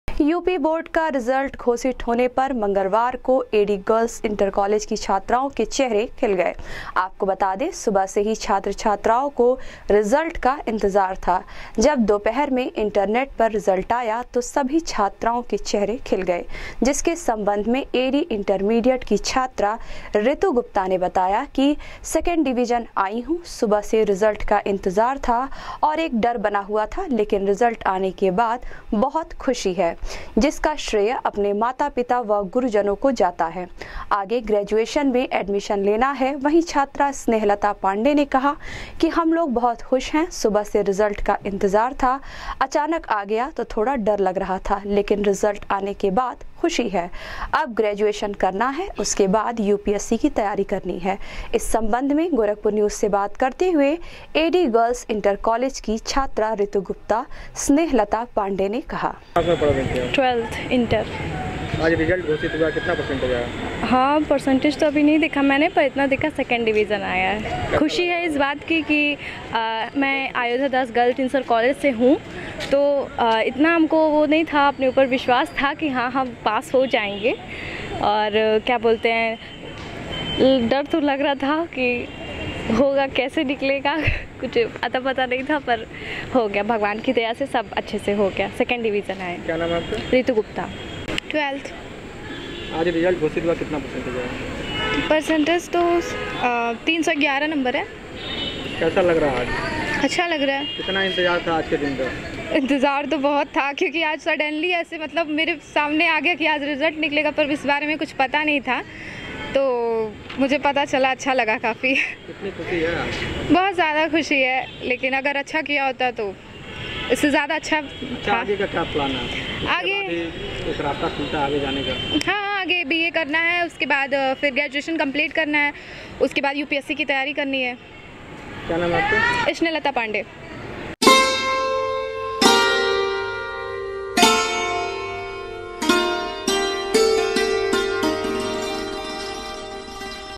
यूपी बोर्ड का रिजल्ट घोषित होने पर एडी गर्ल्स इंटर कॉलेज की छात्राओं के खिले चेहरे, कहा रिजल्ट का
यूपी बोर्ड का रिजल्ट घोषित होने पर एडी गर्ल्स इंटर कॉलेज की छात्राओं के खिले चेहरे, कहा रिजल्ट का सुबह से था इंतजार,सभी लोग हो गये है पास